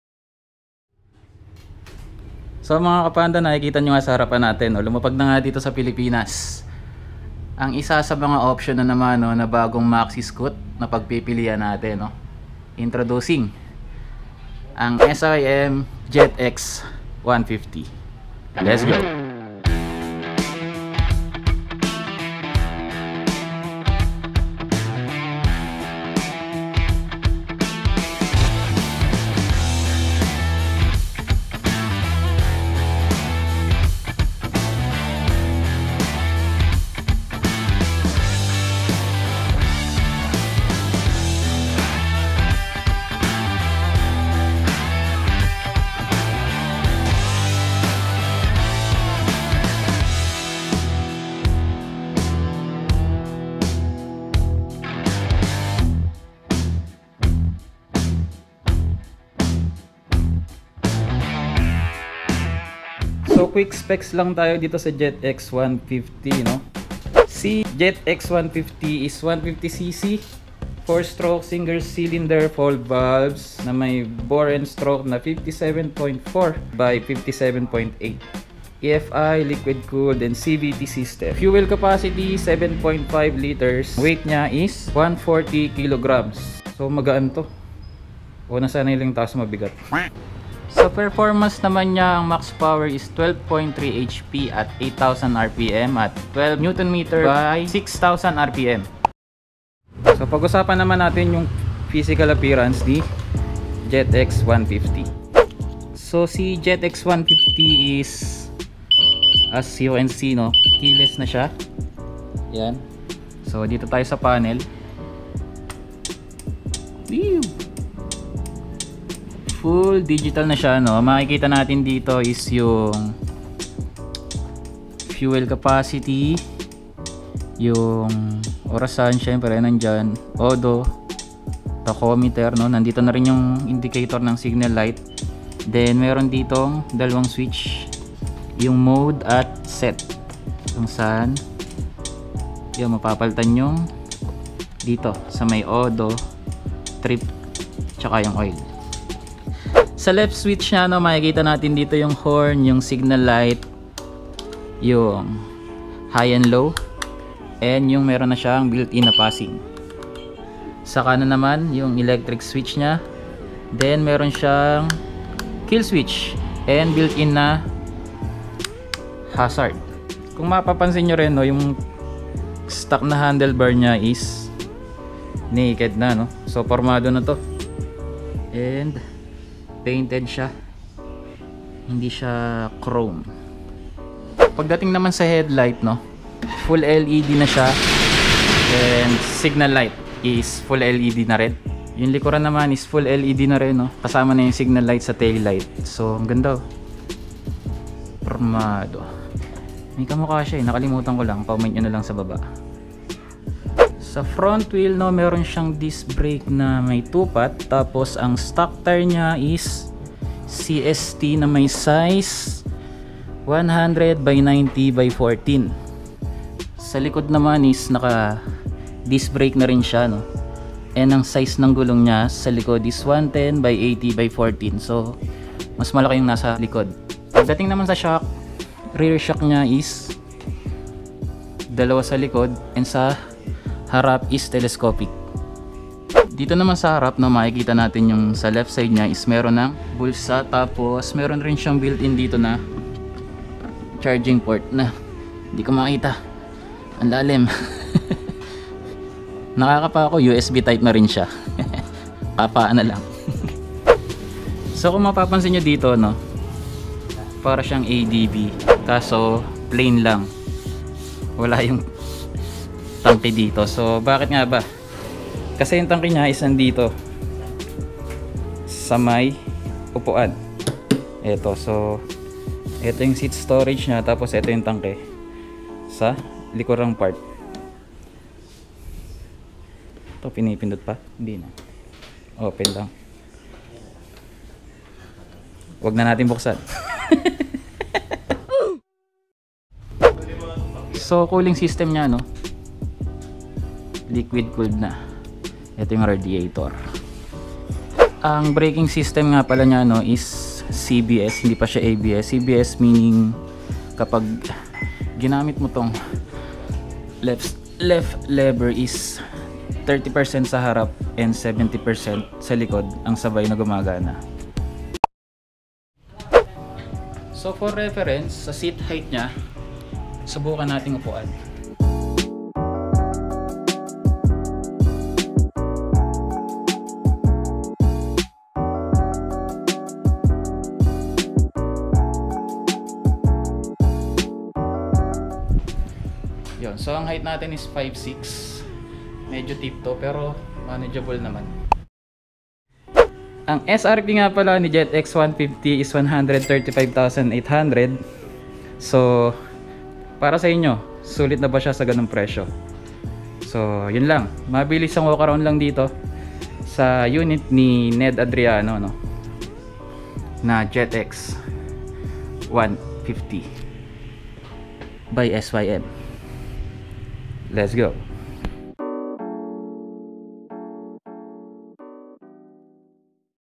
All New SYM Jet X 150 | Sulit kaya? | Review, Specs and Price | Boss Panda
Bagong MaxiScooter Option KaPanda. Sulit kaya?